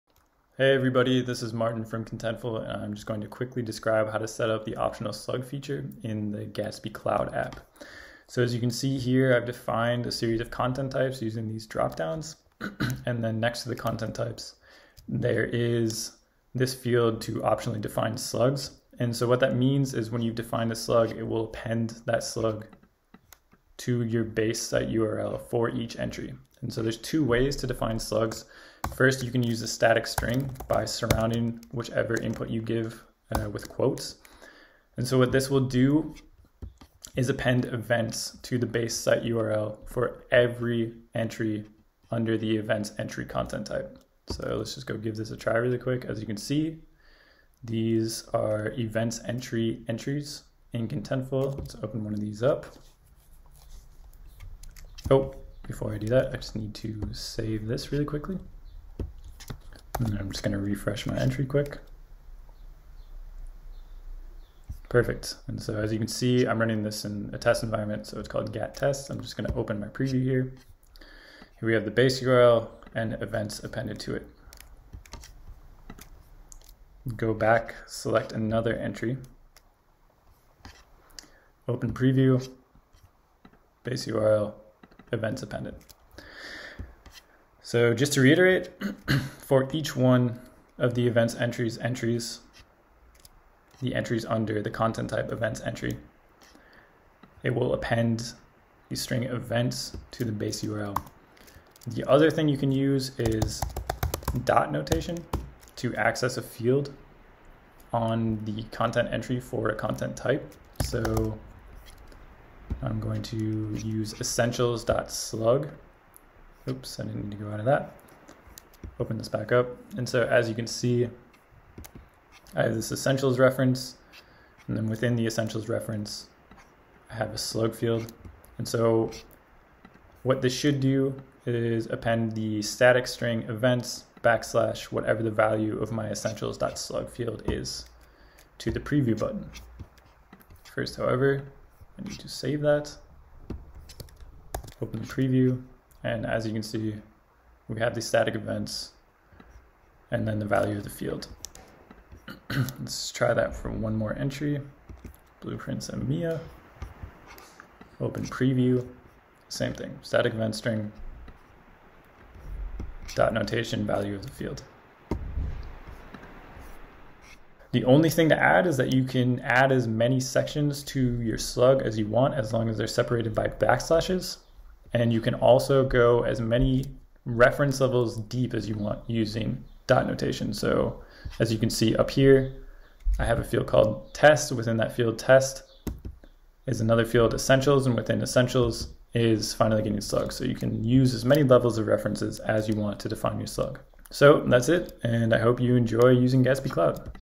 Setup optional slug in gatsby cloud app
How to set up optional slugs in the gatsby cloud app, in contentful.